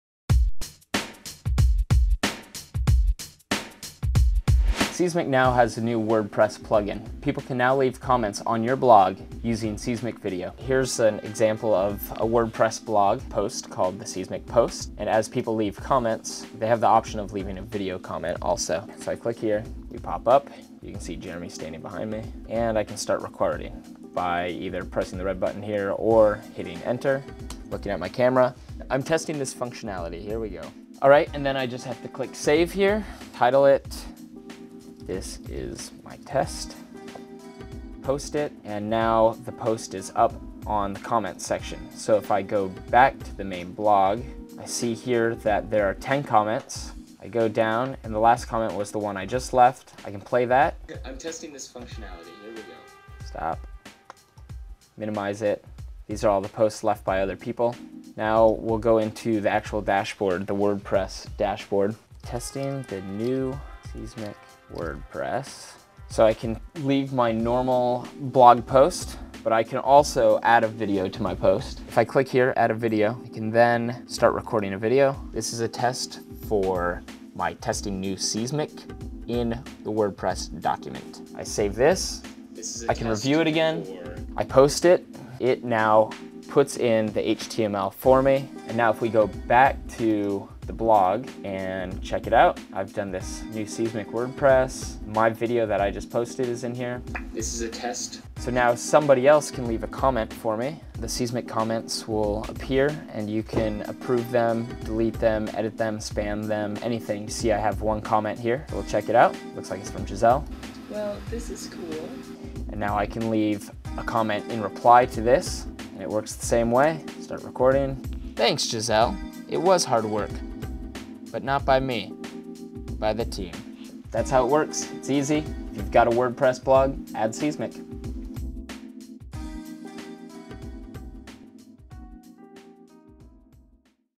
Seesmic du Jour 130: Seesmic WordPress Plug-in
Introducing the Seesmic WordPress video comment plug-in! (@ In today's show, Whit gives a quick demonstration of the new Seesmic plug-in for Wordpress. You can now leave video posts on your WordPress blog and get video comments in response. it's awesome!